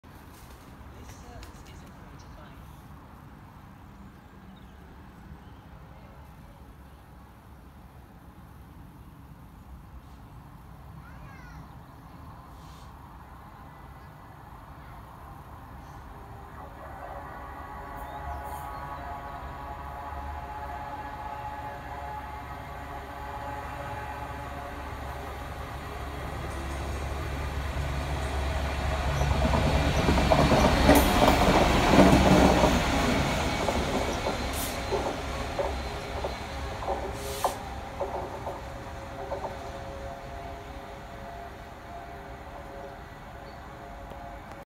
66712 Peterborough power signal box light loco 13:22 running 41 minutes early Lea Road 7/9/2023
66712 Peterborough power signal box light loco 13:22 running 41 minutes early Lea Road 7/9/2023 running as 0E30 Whitemoor Yard L.D.C Gbrf to Doncaster Up Decoy